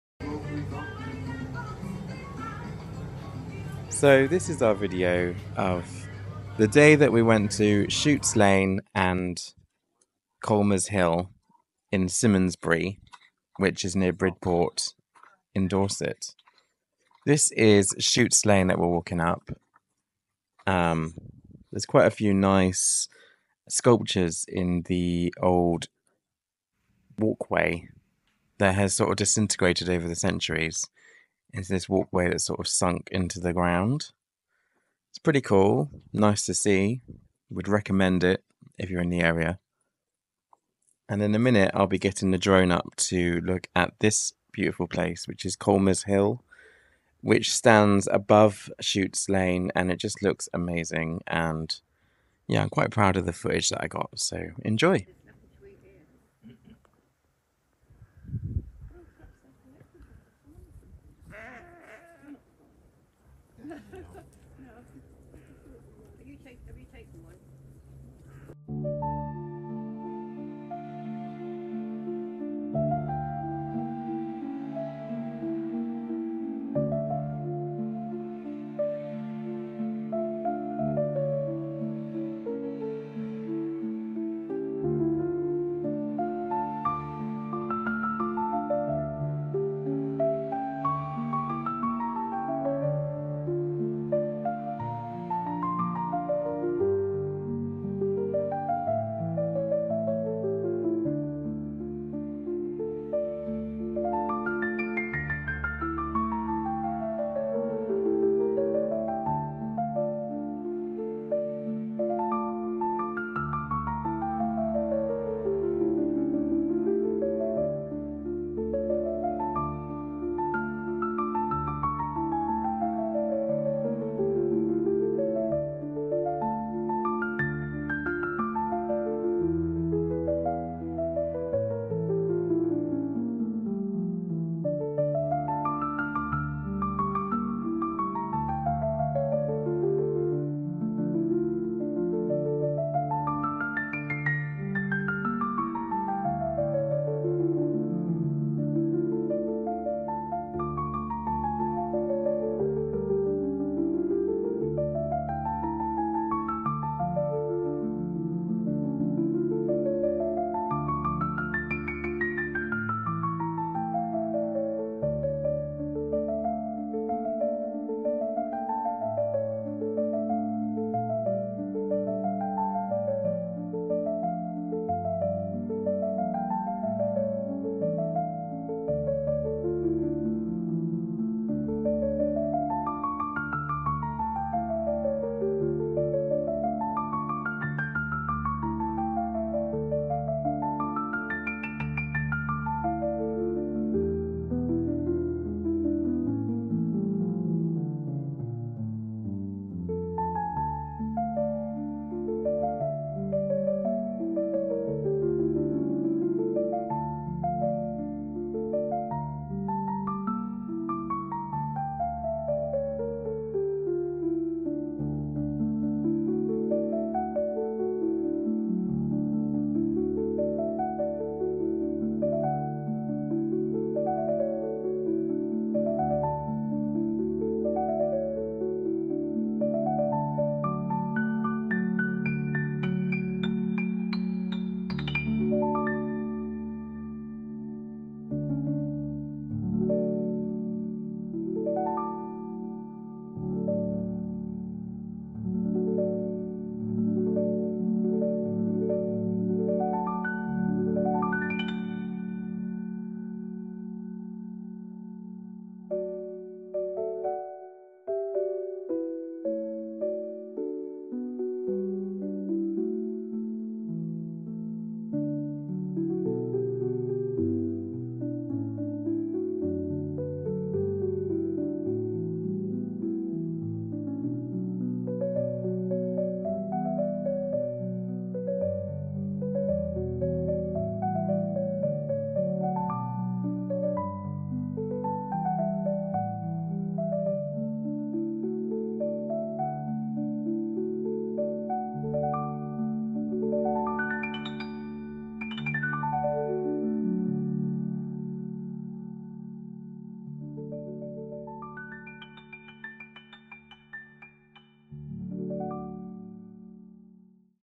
Colmer's Hill
Last week, I and a couple of friends headed to Symondsbury in the heart of Dorset. Here, there are a few sights to see that you wouldn't necessarily know about.

First, there is Shute's Lane... a passage that was formed over centuries by the local villagers transporting stone from the nearby Quarry Hill. The wagons they used caused the soft stone to collapse over time, creating a haunting and eerie walk way, which also has carvings in the stone too. It is truly a sight to behold if you are in the area.

Secondly, and the focus of this video, another great sight is Colmer's Hill, which stands 400 feet above the nearby area. It is a very picturesque hill and offers some truly amazing sights of Dorset from the top of it.

Thanks!